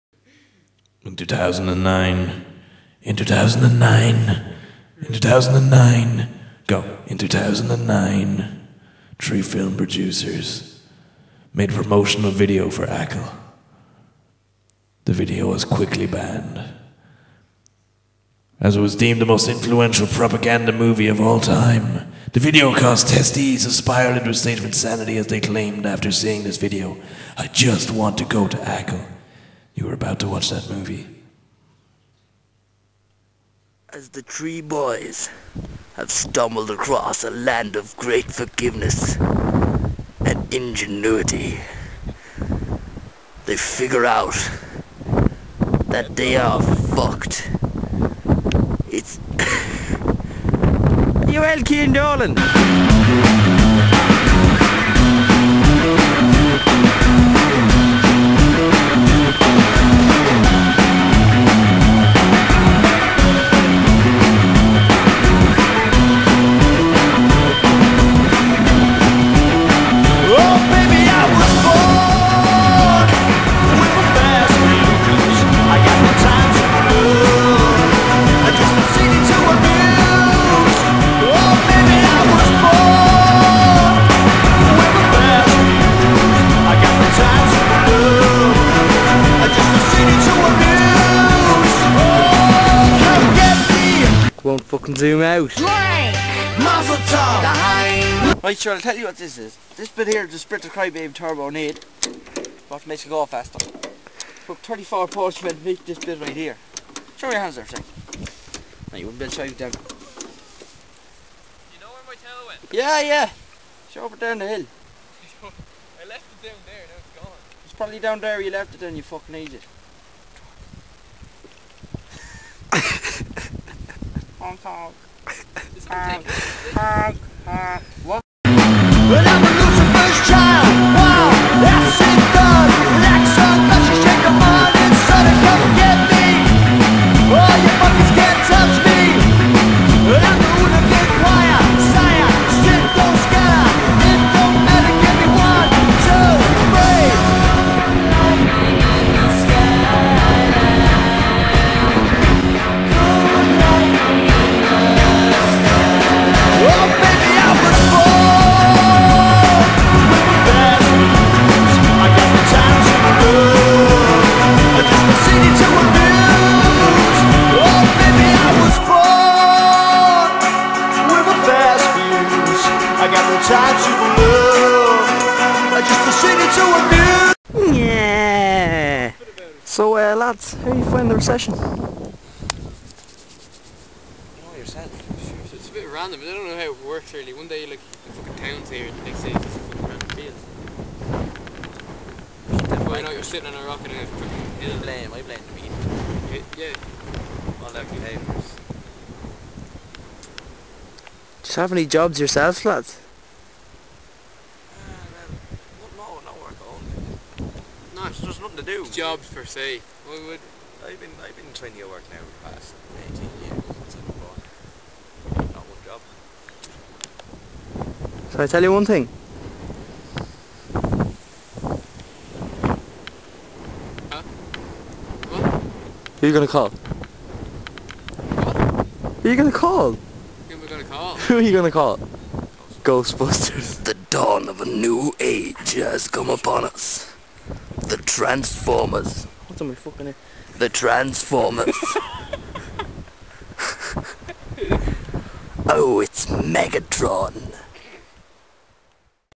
Got Acaill?
In 2009 three film producers made a promotional video for Acaill island that was so influential it caused teetees to spiral into a state of insanity claiming, "after seeing this video, I just want to go to ACAILL!". You are about to watch that movie...